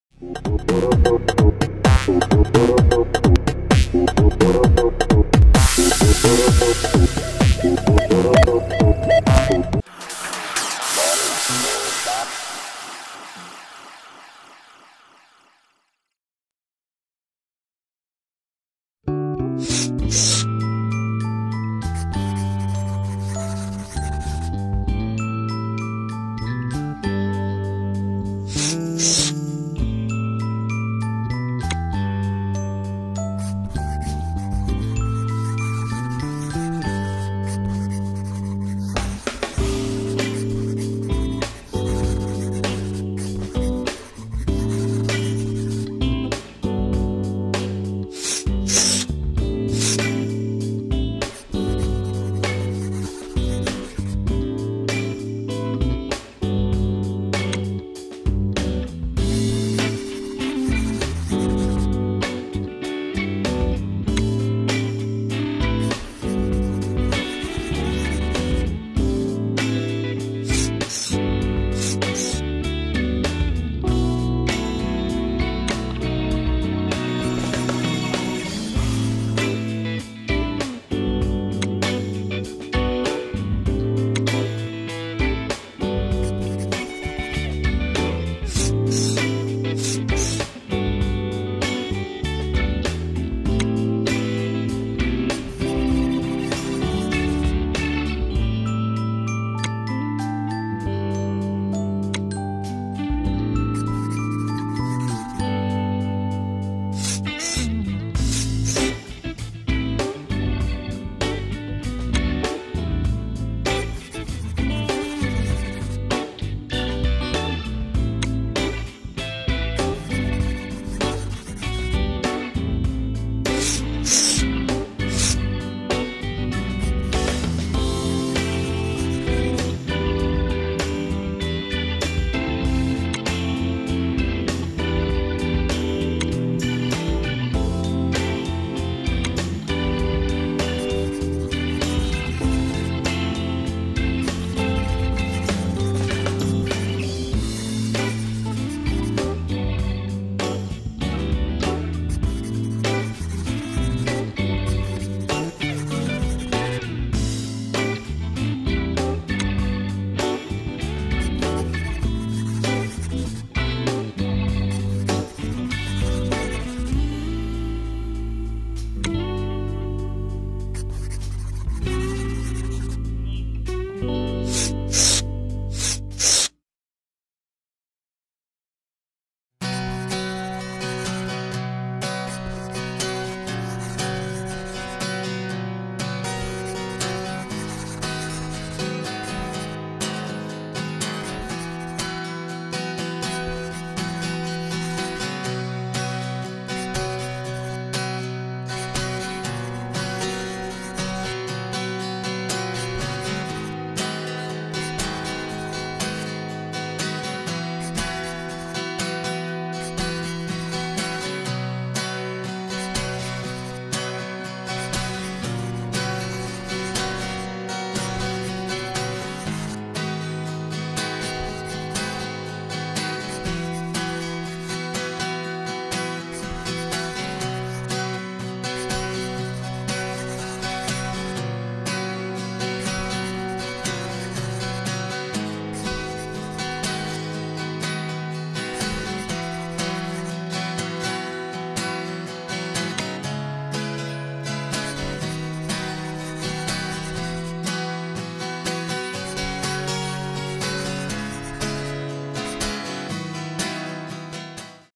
MAVIB (Multimedia Audio Visual and Broadcasting)
Sebuah Hasil Karya Mahasiswa Raharja dengan Judul
"Apa itu Jurusan Teknik Informatika konsentrasi Multimedia Audio Visual and Broadcesting ? "

Open House : Senin sd Jumat 08:00 sd 20:00 (Sabtu 09:00 sd 14:00)